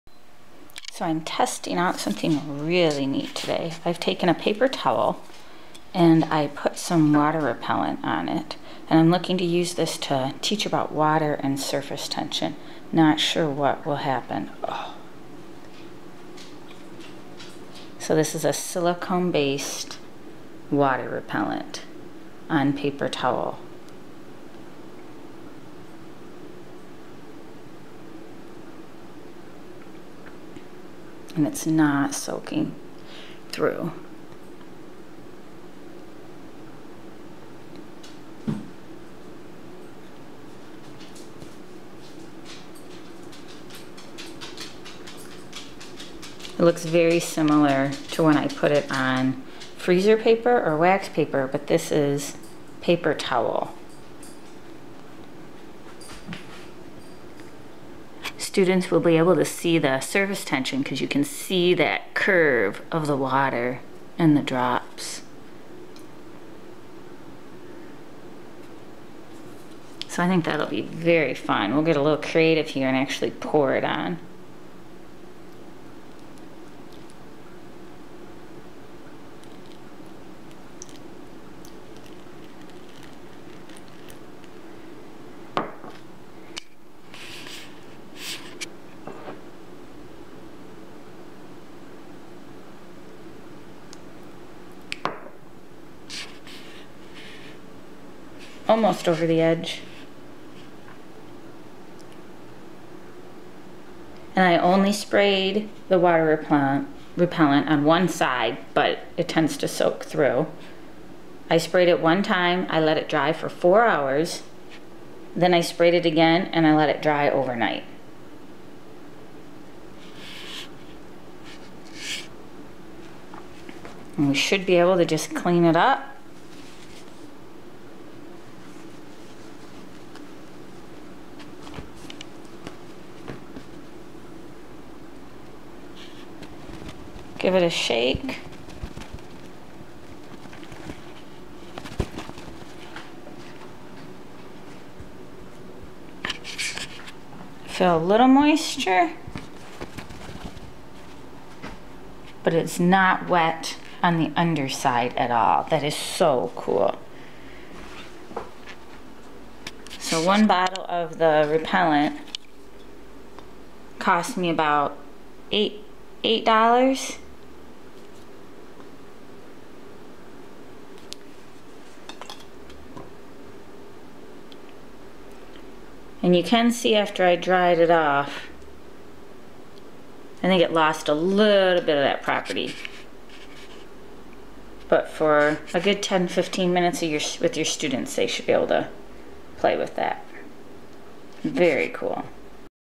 Water Proof Paper Towel
What happens when you make water proof paper towel?  Here are the directions for using silicone based water repellent on paper towel.  This is a great way to teach about hydrophobic and hydrophilic substances.  For more visit kimberlyscottscience.blogspot.com